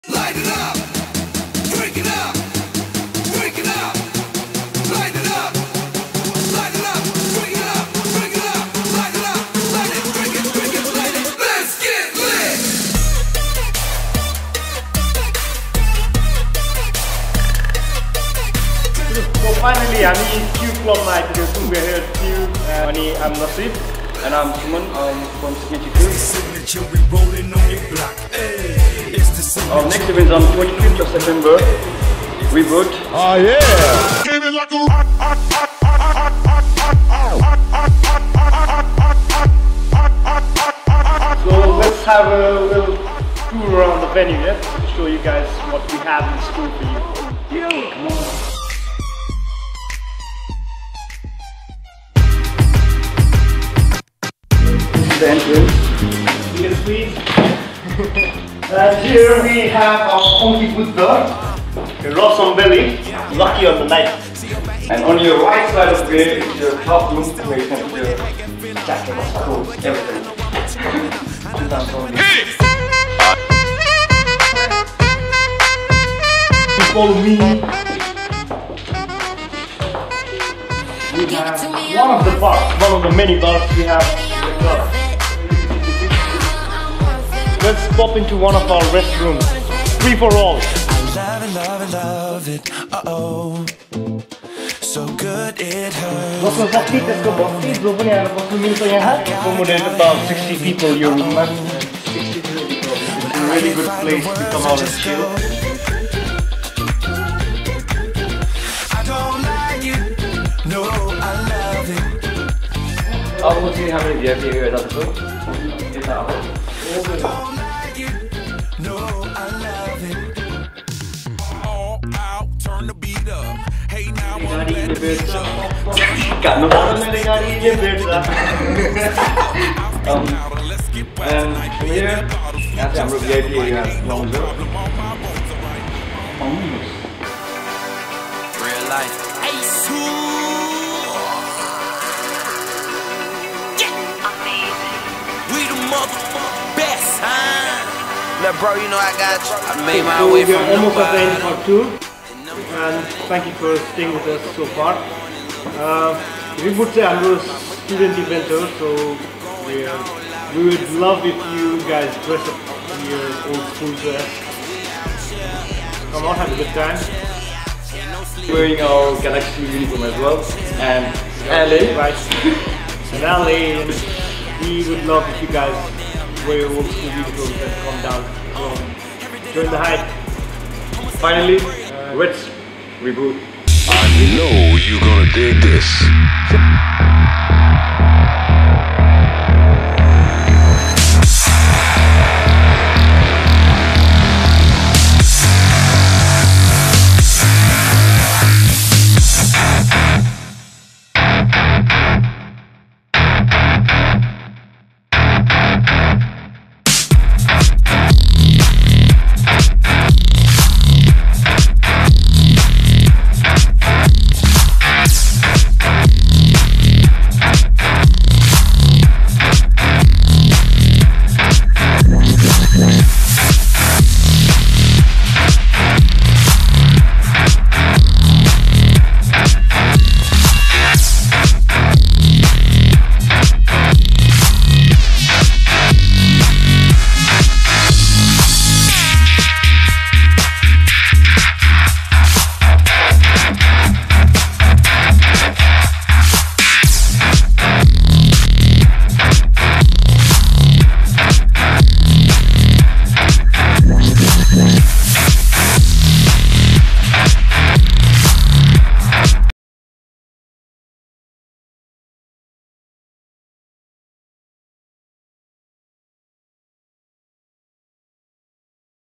RE:BOOT 2015 - Qube Projects VLog
This year the crew took a radical decision to replace our much loved summer installment, 'The Show'. Since the big boots of expectation could only be filled with something extraordinary, we had to reach deep and really pull a rabbit out of the hat. So carying down the memory lane, we bring you an event that has shaped and defined our nights and events. A Five Year in the making, the event that single-handedly brought together and established the 'Signaturee Crew' of today. This is an event really not to be missed. As the old saying goes:

So come join us while we take you a a tour around 'The Qube Projects'